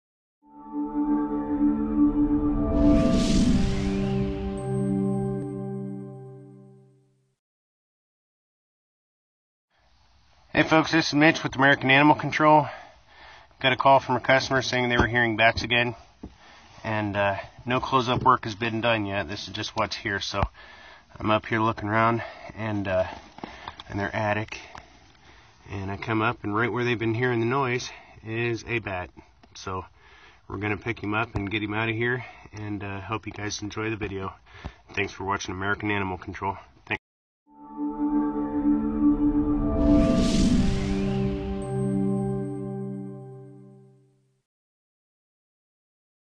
Bats in the Attic - Crown Point, Indiana
Our customer in Crown Point, Indiana called us with bats in their attic.  They’re hearing noises up there a lot.

We haven’t performed any of the close up work yet, and this is what’s here.  Our technician came up to inspect the attic, and right where they’ve been hearing the noises, he found a bat.